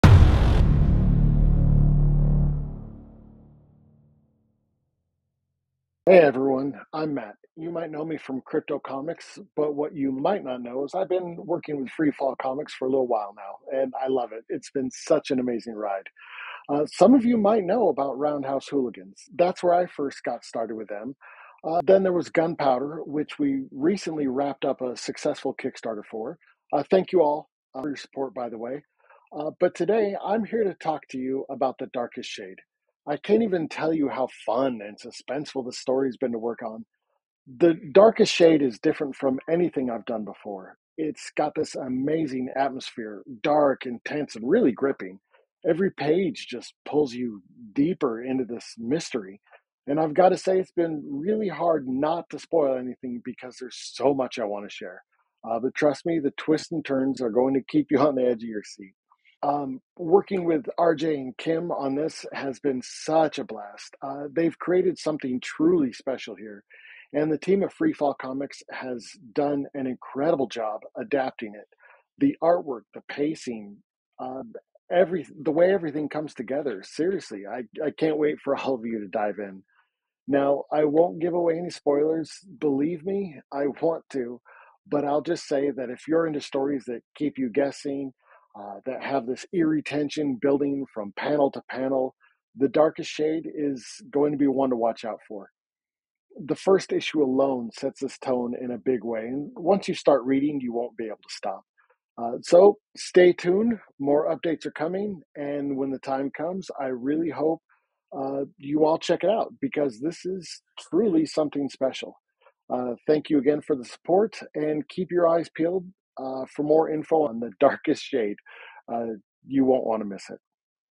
A NEW HORROR SERIES
Our talented lettering artist and good friend, M. Scott Russell shares his experience working on our upcoming cosmic horror series, The Darkest Shade, and what the project means to him personally.

The Darkest Shade Book 1
Two parents, torn from their dream life by their daughter's mysterious death, are lured into a nightmarish realm by an ominous figure.